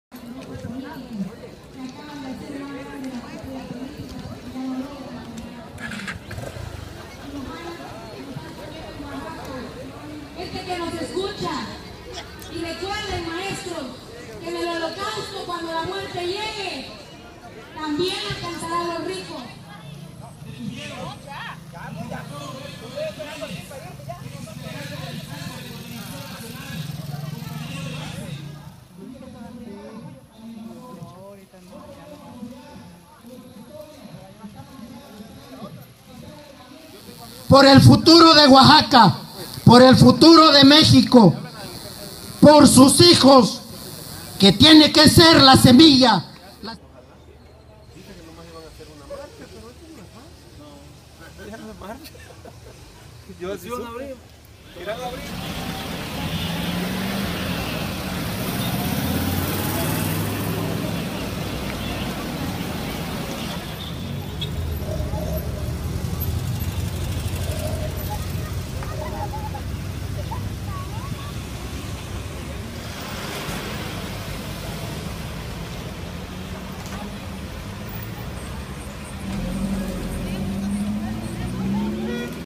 Bloqueo carretero por la sección 22 en Pinotepa
El pasado jueves un grupo de más de 200 profesores bloquearon la vía federal 200 para exigir la liberación de 4 de sus líderes.